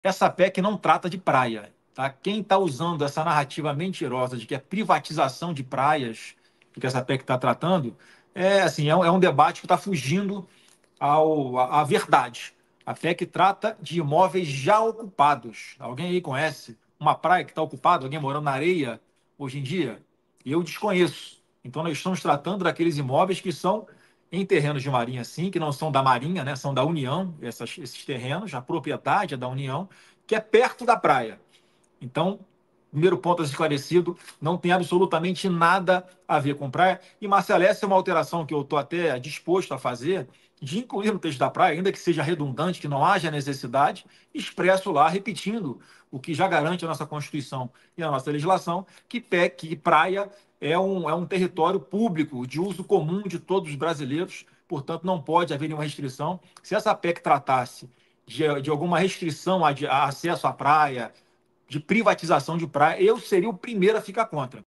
A PEC 3/22 NÃO PRIVATIZA PRAIAS!
Não acredito em mentiras da esquerda. A PEC sequer trata de praia.